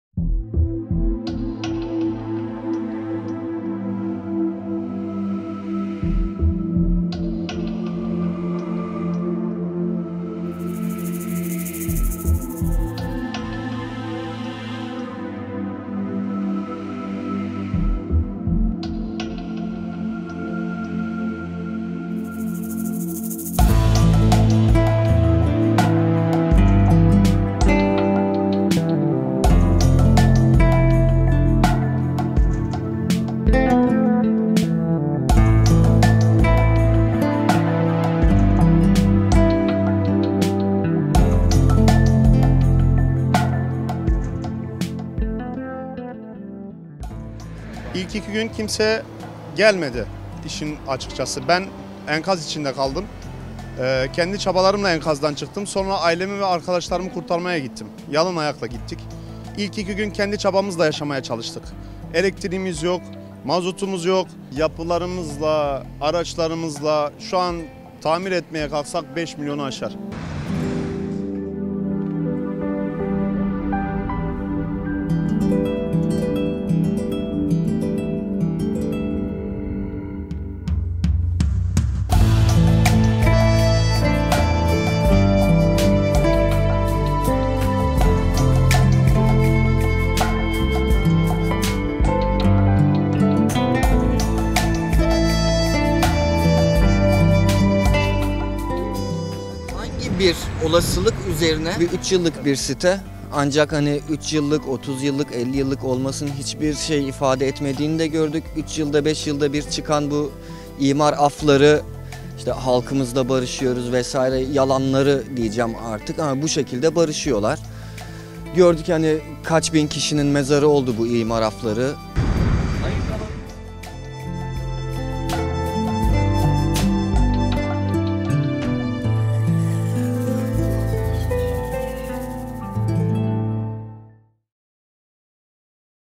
Antakya quake victims slam Turkish government negligence
Antakya, the once-thriving city of civilizations now lies in ruins after it was shaken in the February 6 earthquake. More than three weeks on, quake victims in the city accuse the government of being negligent and failing to come to their aid.